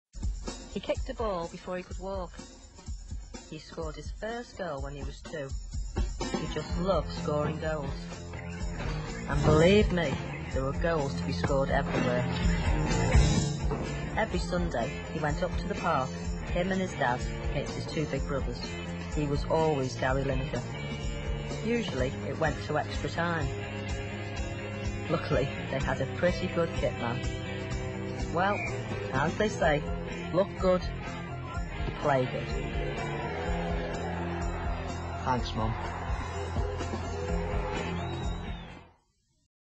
Michael Owen - Persil Ad - 2004
Michael Owen appearing in an ad for Persil in 2004.